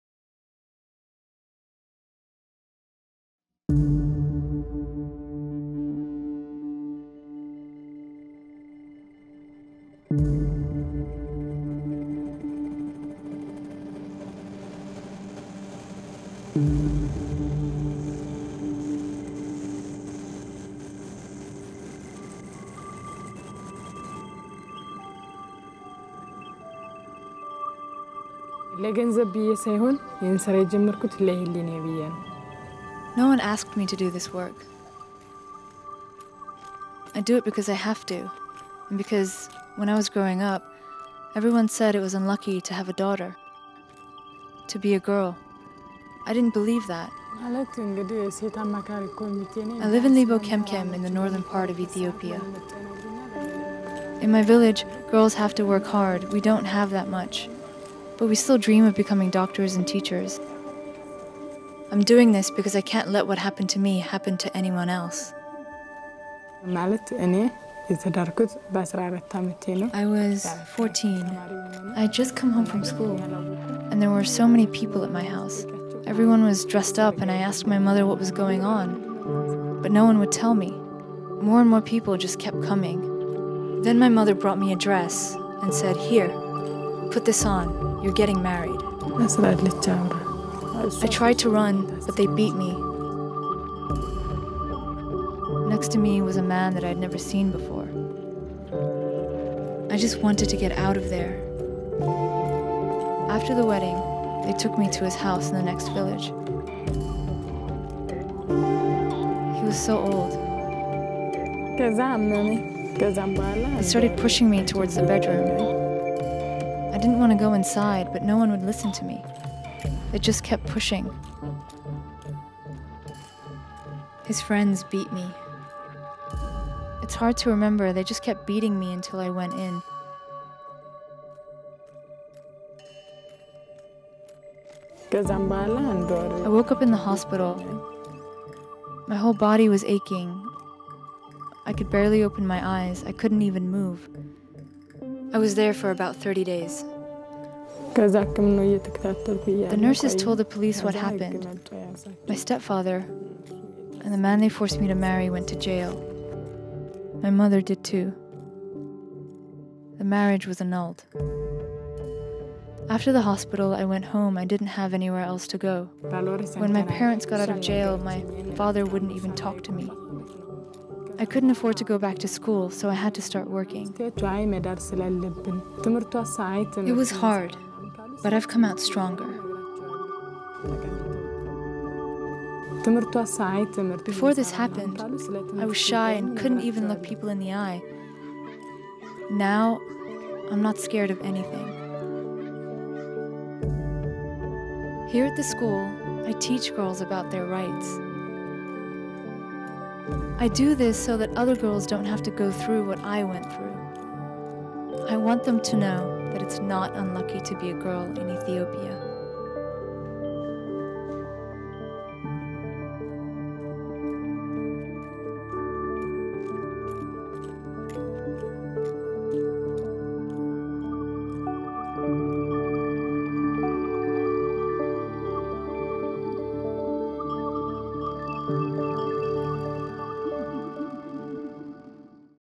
Standing Up To Early Marriage: Melka
"I want girls to know there are people out there like me who will fight against early marriage." - Melka from Libo Kemkem, Ethiopia on standing up against forced marriage. 10x10 produced this video for Girl Up.